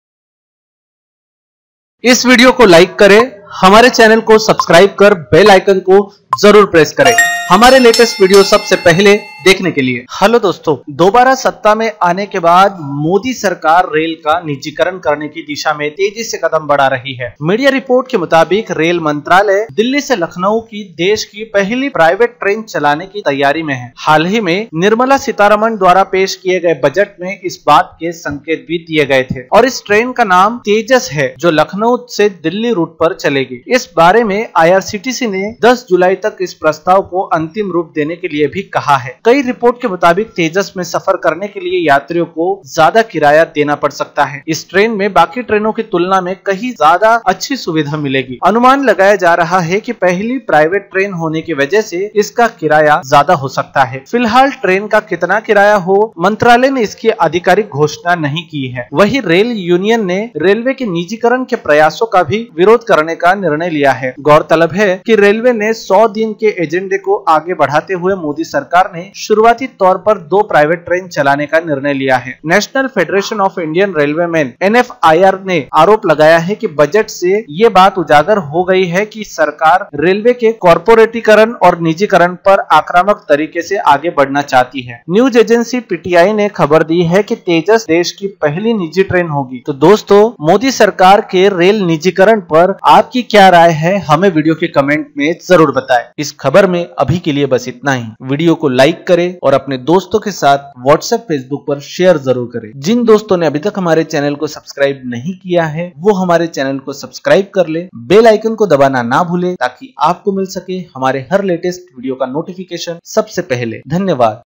Railway Employees की धमकियों के बावजूद, Tejas Express First Private train हो सकती है।
BB News : Railway Employees की धमकियों के बावजूद, Tejas Express First Private train हो सकती है

Delhi-Lucknow Tejas Express to be India's first train operated by private sector. The Tejas Express (train number 12585) will depart from Lucknow Junction at 6.50 am and reach New Delhi at 1.35 pm. The Railways, despite protests from its unions, is going ahead with its 100-day agenda of handing over operations of two of its trains to the private sector.

BB NEWS!!